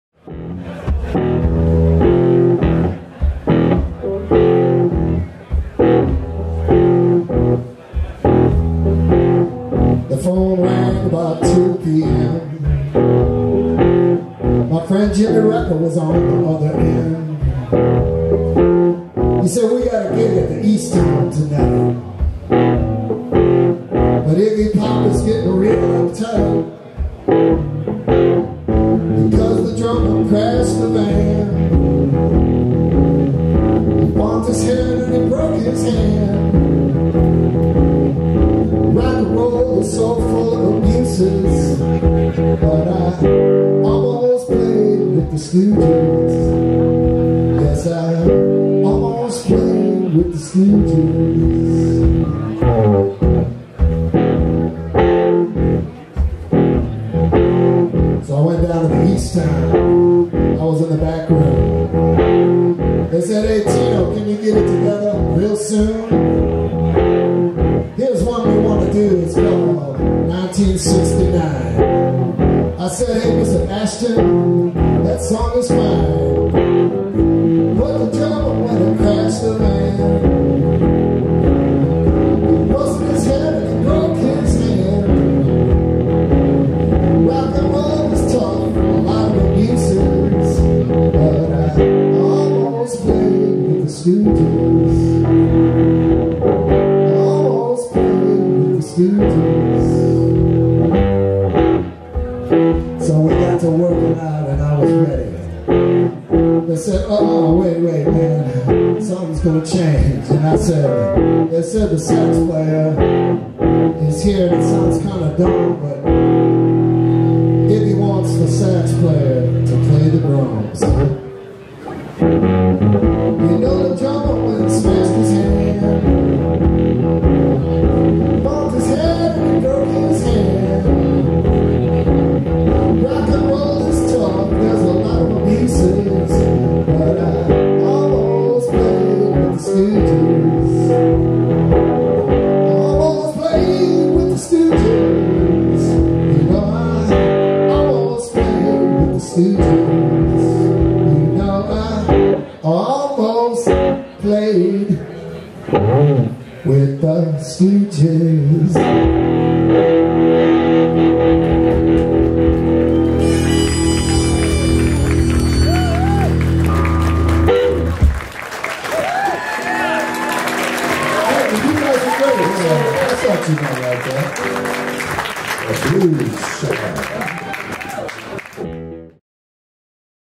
''ALMOST PLAYED WITH THE STOOGES'' - TINO GROSS @ Callahan's, Aug 9, 2015
Tino Gross brings his blues dumpster show to Callahan's Music Hall; Aug 9, 2015.  Filmed in 1080HD